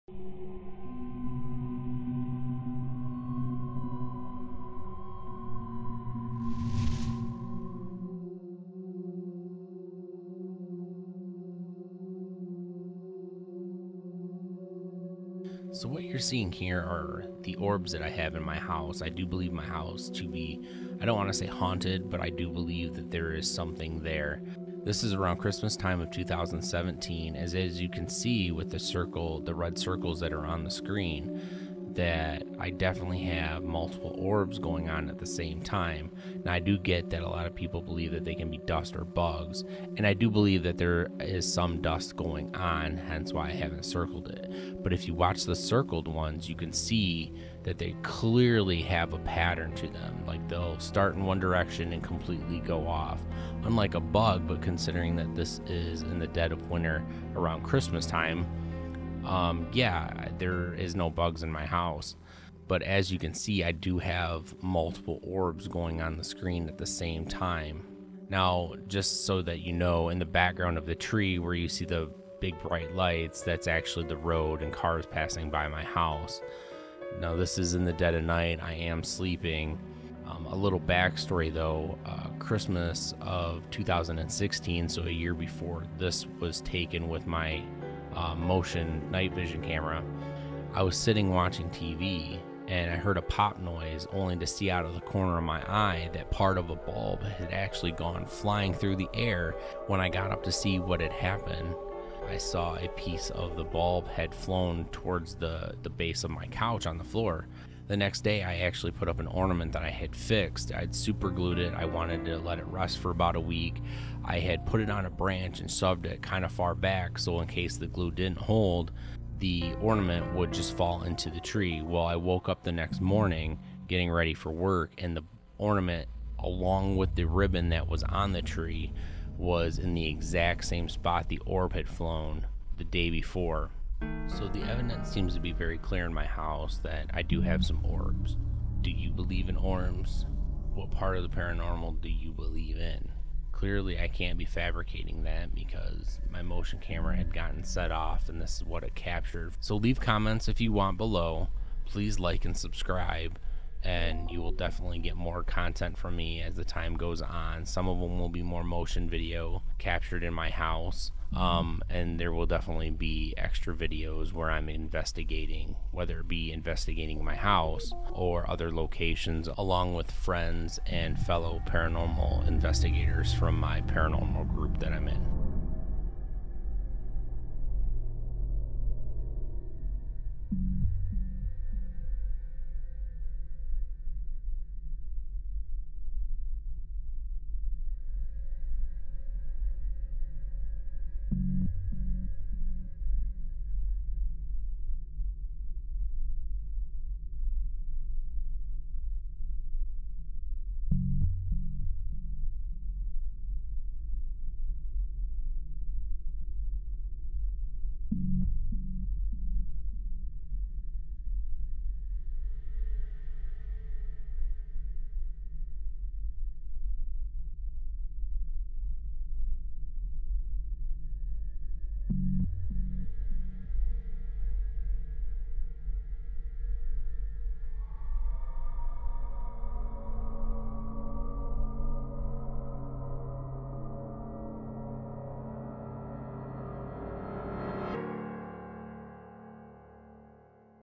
Ghostly Hauntings: Orbs - House Paranormal Evidence 1
This is the first of what I can say will be many of the orbs I capture in my house using my night vision motion camera.

Whether you believe or not... I believe orbs exist and I have many in my house.